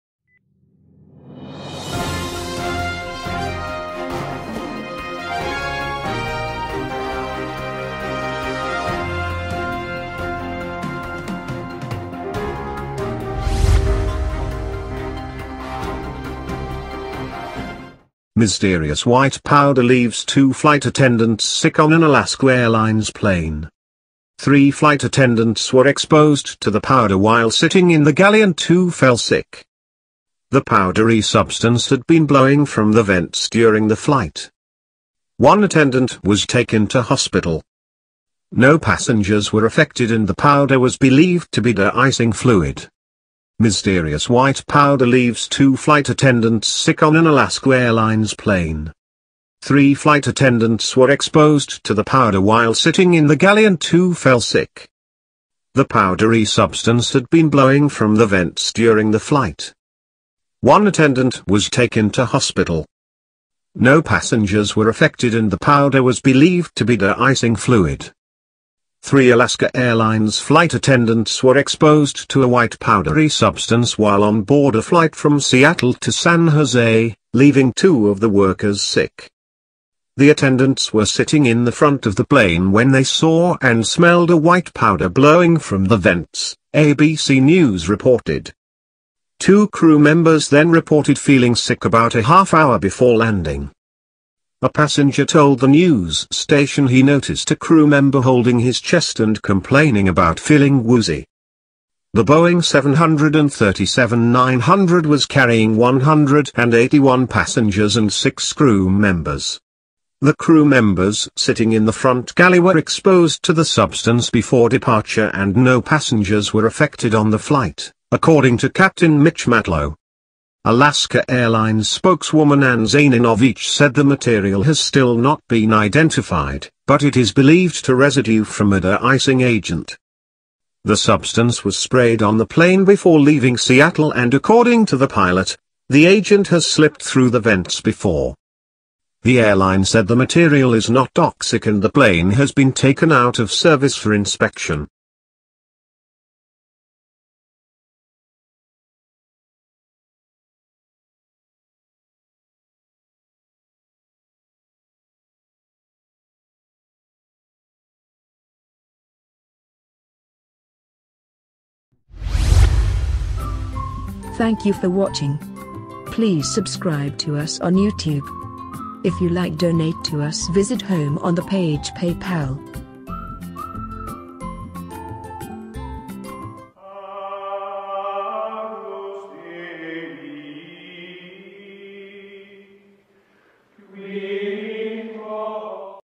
Mysterious white powder leaves two flight attendants sick on an Alaska Airlines plane
Three flight attendants were exposed to the  powder while sitting in the galley and two fell sick
The powdery substance had been blowing from the vents during the flight
One attendant was taken to hospital
No passengers were affected and the powder was believed to be de-icing fluid.